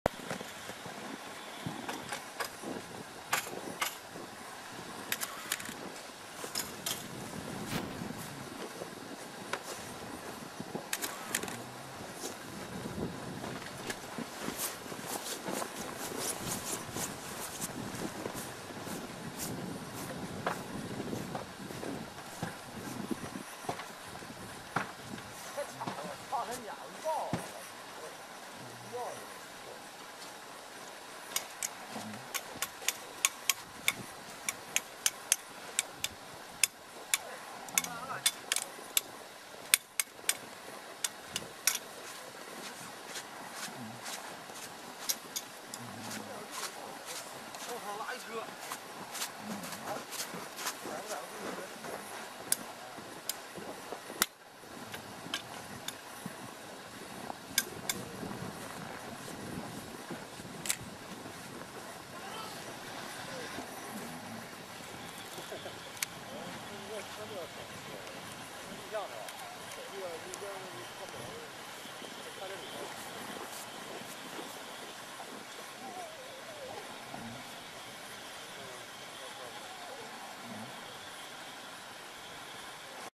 Steam of Weihe Forest Railway China(Feb.2003) 3 中国・葦河森林鉄道の蒸気機関車（2003年2月）3
2003年2月撮影。中国・葦河森林鉄道のC2です。